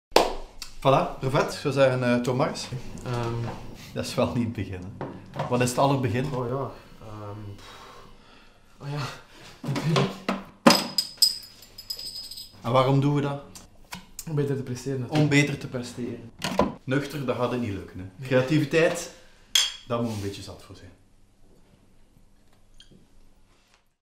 Camera brevet halen op Narafi - Talkshow uitsmijter
Hoe haal je gemakkelijk je camera brevet op narafi in 20 seconden.